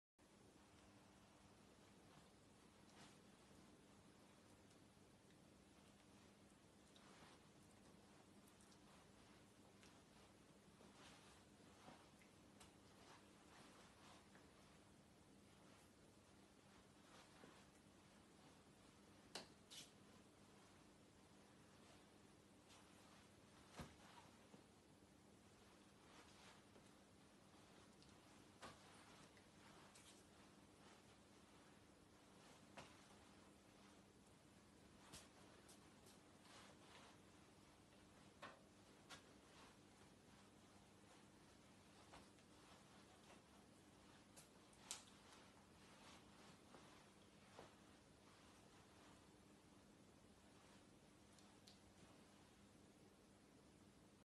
Receiving your COVID-19 vaccination - British Sign Language (BSL)
Using British Sign Language, Lead pharmacist for Central North Leeds Primary Care Network, Fazila, explains what to expect from your coronavirus vaccination appointment after being invited by the NHS.